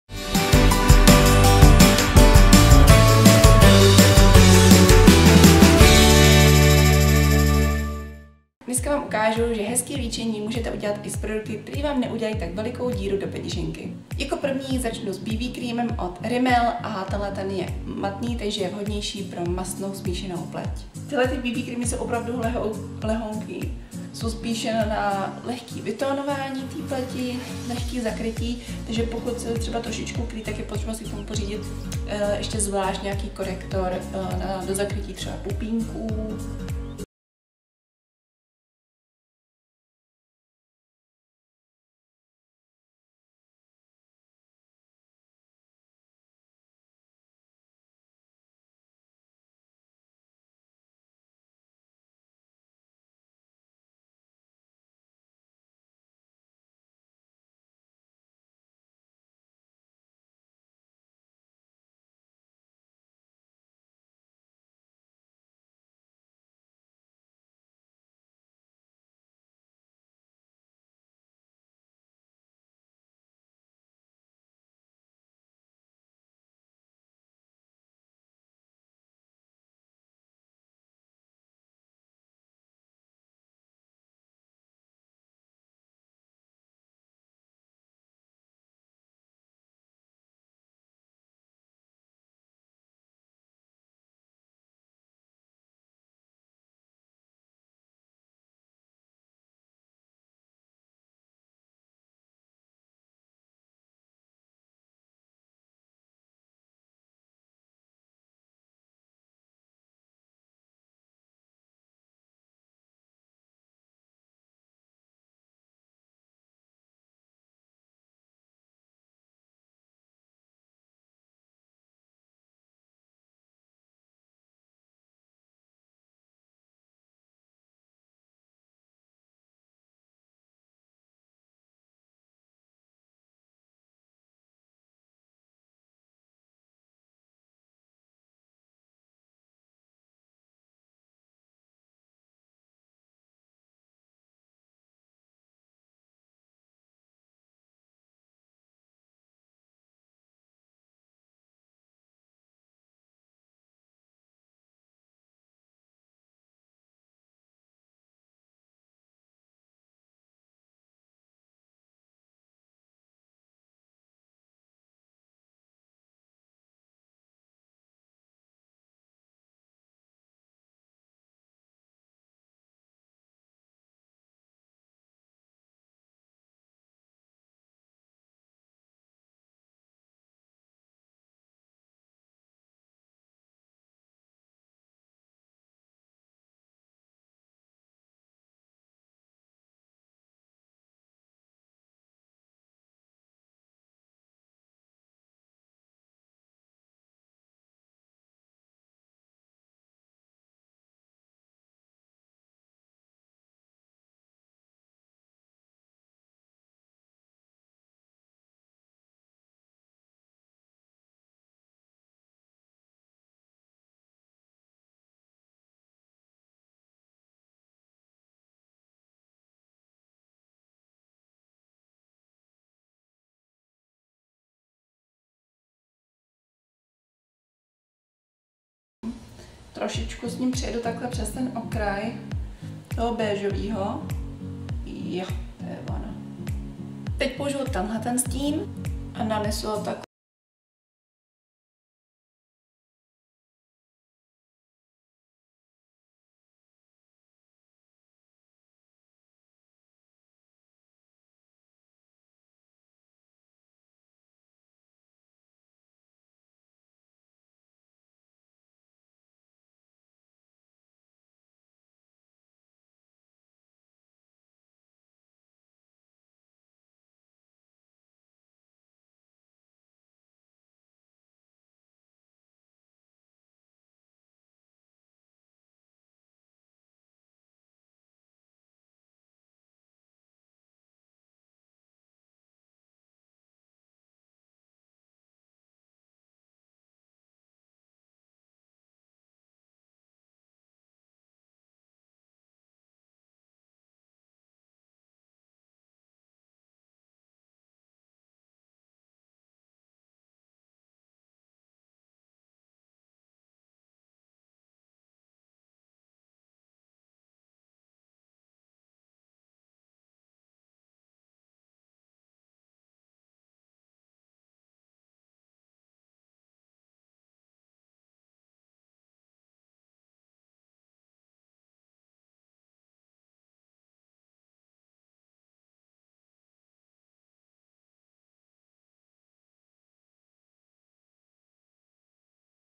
QUICK & CHEAP make-up tutorial | Rychlé a levné denní líčení do školy či do práce
Pro vytvoření bezchybného každodenního looku není vždy potřeba utrácet za produkty tisíce. I s dekorativní kosmetikou, která stojí mnohdy méně než 100 Kč, lze vytvořit stylový  make-up, který hravě zvládne každá z vás.

POUŽITÉ PRODUKTY:

BB krém Rimmel London BB Cream Matte 9in1 SPF15 (cena: 96 Kč)

Dekorativní kazeta Makeup Revolution London Euphoria Palette Eyeshades & Contouring (cena: 101 Kč)

*Uvedené ceny produktů se mohou lišit s ohledem na aktuální skladovost.